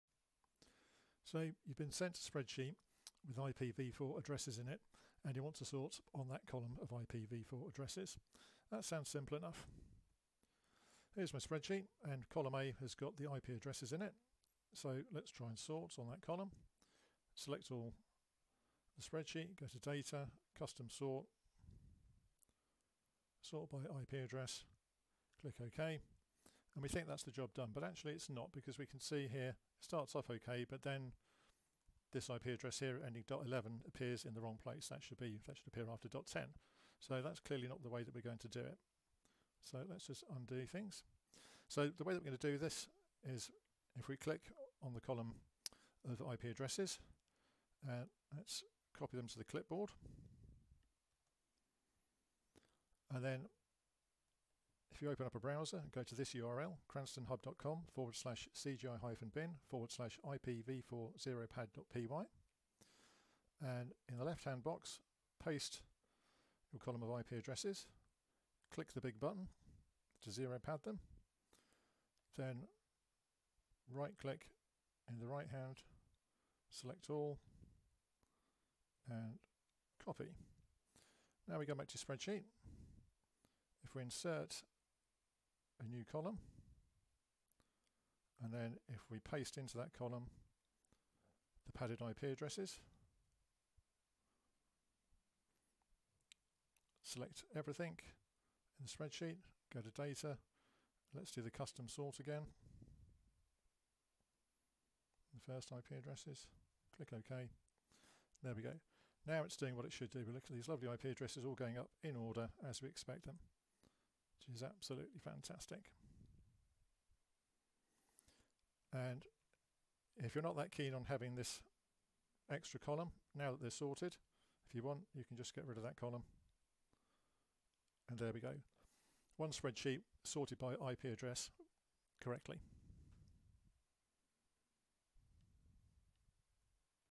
Sorting by IPv4 addresses in Excel
This is one of many ways you can sort by IPv4 address in Excel. By using a webpage to convert the IP addresses to a zero padded format you avoid having to remember complicated Excel formulas or going through a long series of point and clicks to split the IP addresses into seperate columns. Enjoy!